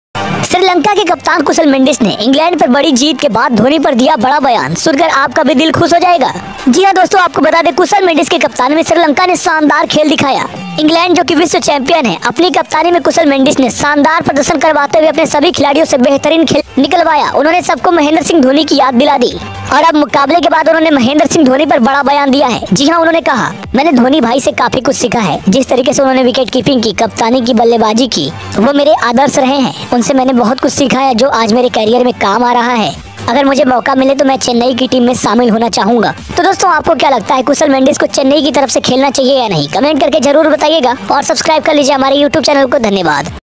Watch Kusal Mendis Heart Winning Statement On Dhoni & after SL Win vs ENG | CWC
credit : icc & HOTSTAR